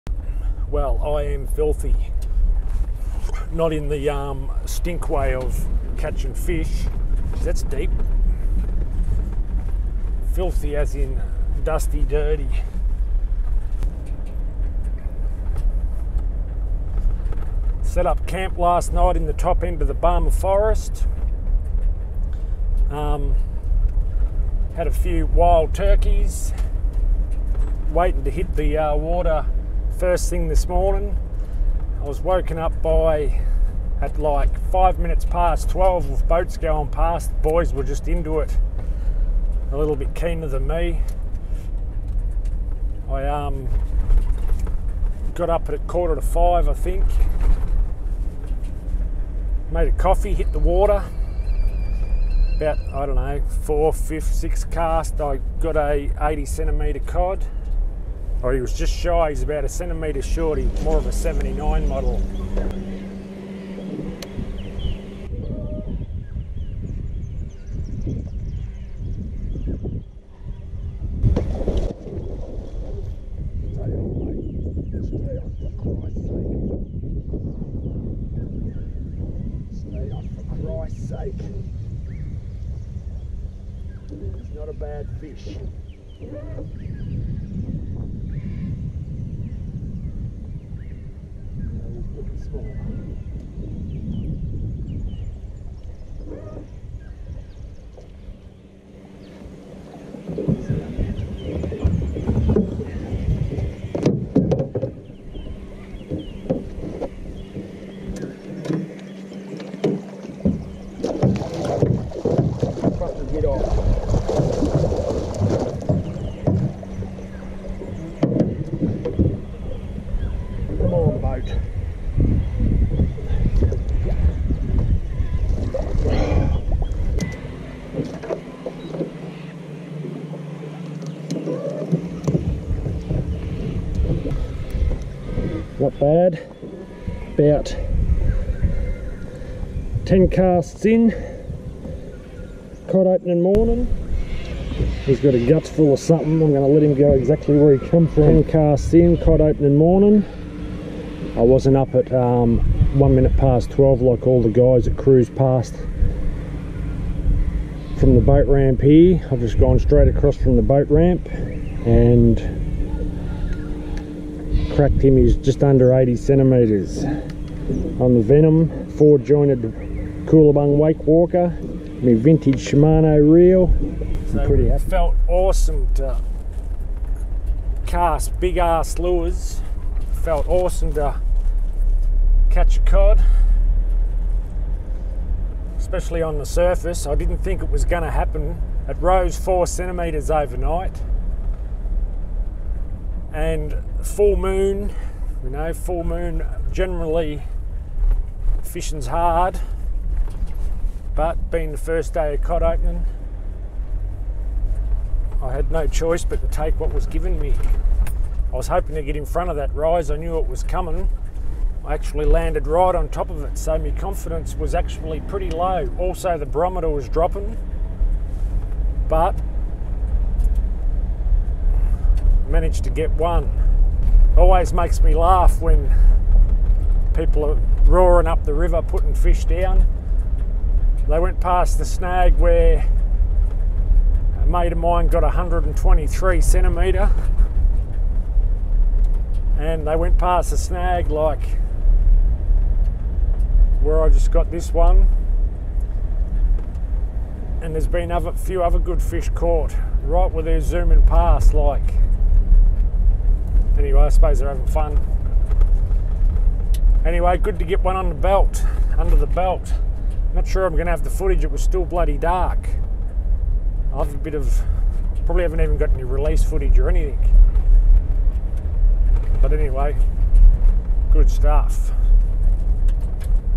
Day 1, 20-21 MurrayCod Season
Ten casts into Day One of the 20-21 Murray Cod  and the 4 jointed Koolabung Wake walker found my first for the new season, always happy to get a run on the board  opening Day :)